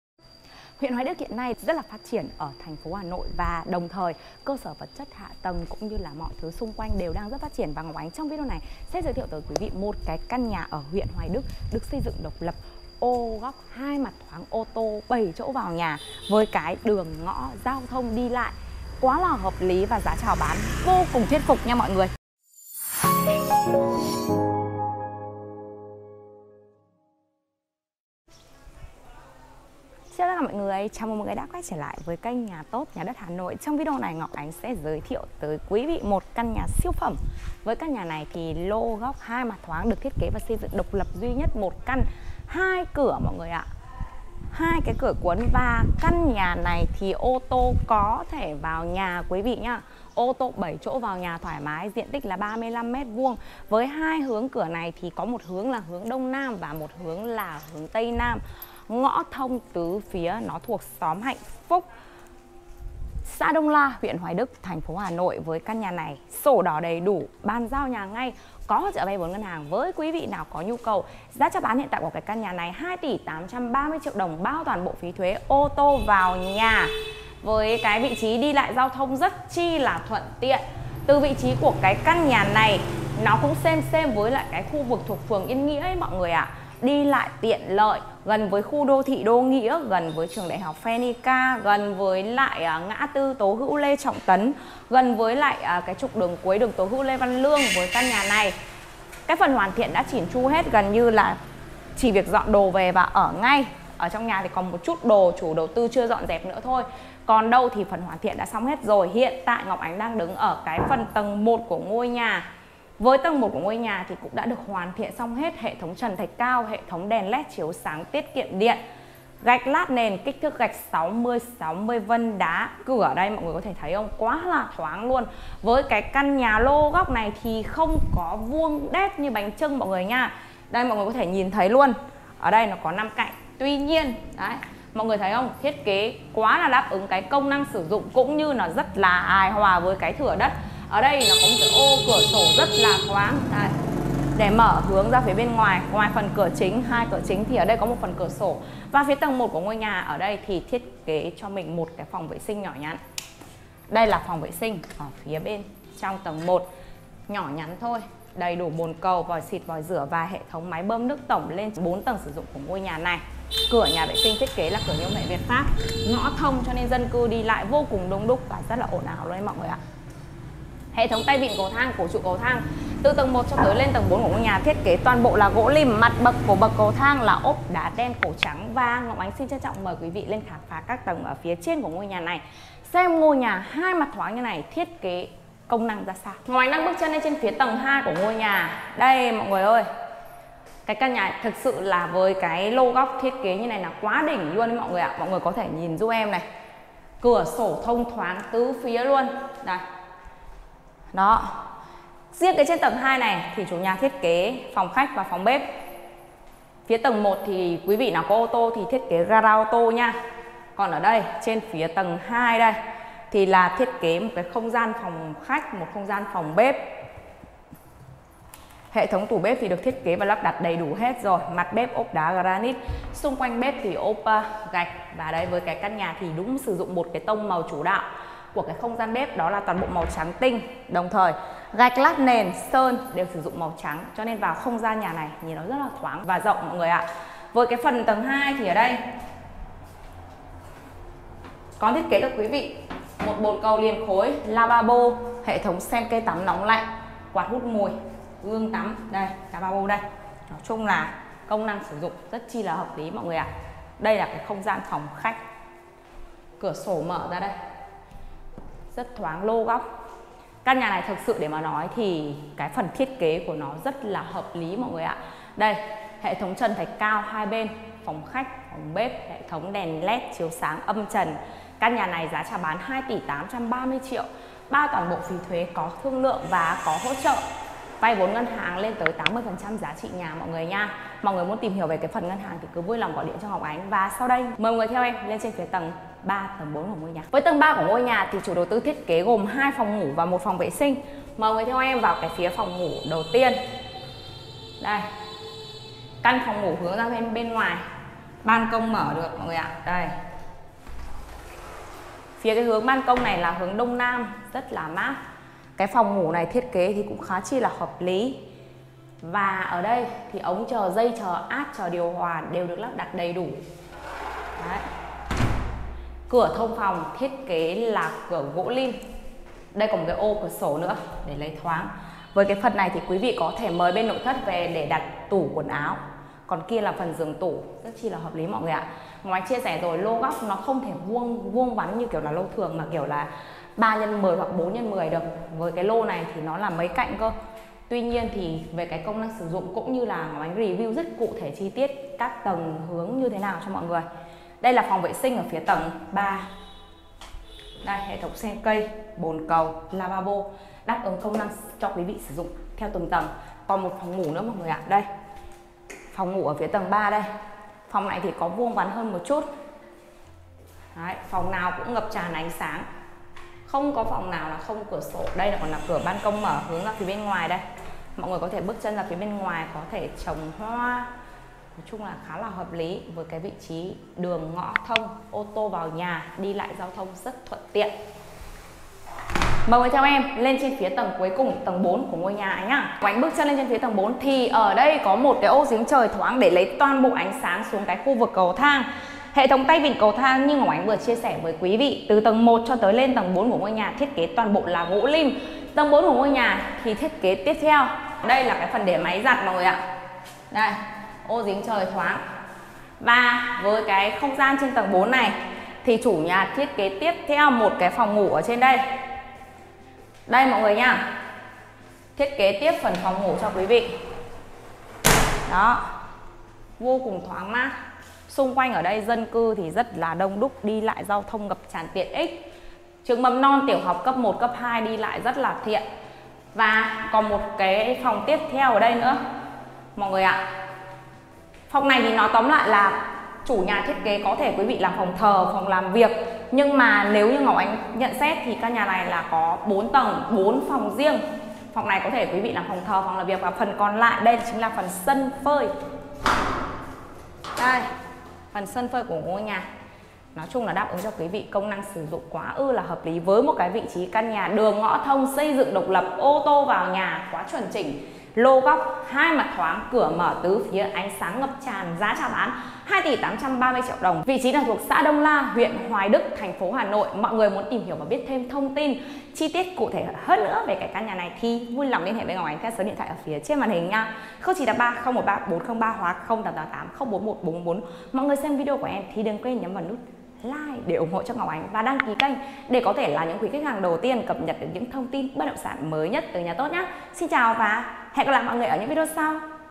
Nhà ở đâu có ga-ra oto giá rẻ như này? Gần trung tâm 4 tầng lô góc | nhà tốt
- Địa chỉ: Đông La, Hoài Đức, Hà Nội
- Diện tích: 35m2
- Hướng nhà: Đông Nam
- Thiết kế: 4 tầng, 3PN, phòng khách, bếp, phòng thờ, sân phơi, 3wc
- Giá bán: 2,83 tỷ